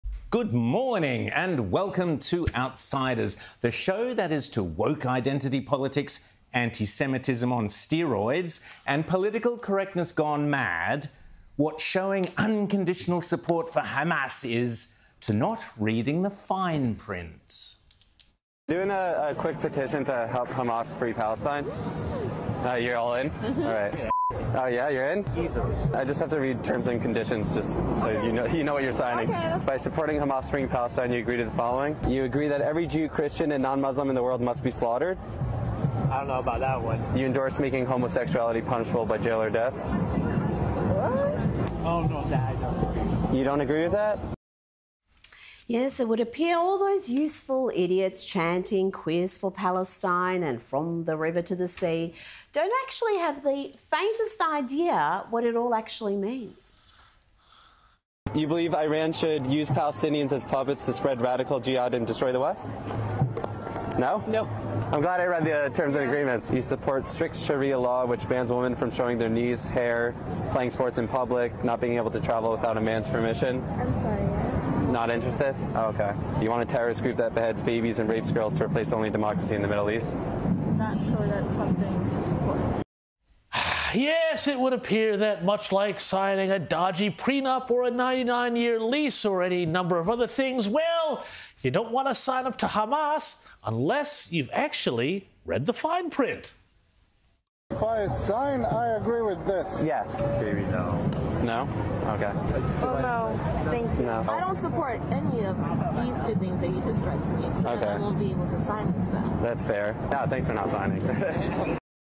‘Read the fine print’: James Morrow praises viral video exposing Hamas' supporters hypocrisy
Sky News host James Morrow has praised a viral video by social media influencer David Atherton in which he asked strangers to sign a petition supporting Hamas’ terrorist actions before revealing the “terms and conditions”.

Several of the strangers appeared happy and willing to sign the petition in support of the terrorist group before Mr Atherton revealed the “fine print”.

Mr Atherton went on to say if you support this petition ‘you agree that every Jew, Christian and non-Muslim in the world must be slaughtered’.

The strangers immediately backtracked and refused to sign and Mr Morrow has praised the video for highlighting the hypocrisy of pro-Palestine and Hamas supporters.

“Much like signing a dodgy prenup or a 99-year lease or any number of things, well you don’t want to sign up to Hamas unless you’ve actually read the fine print,” he said.